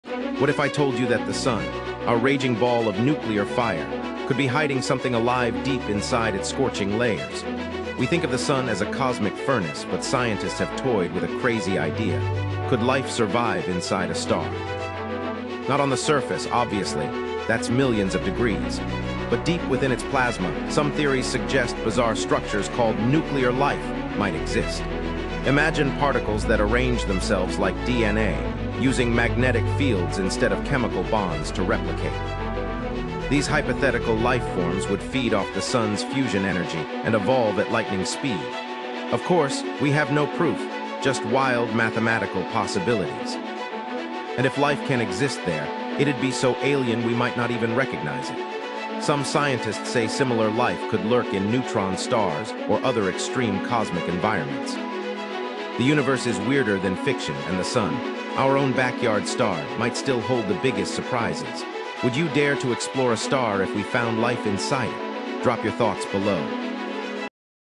Could Life Actually Exist Deep Inside Our Raging Sun?
Is it possible for life to exist inside a star? The mind-blowing science behind this wild theory!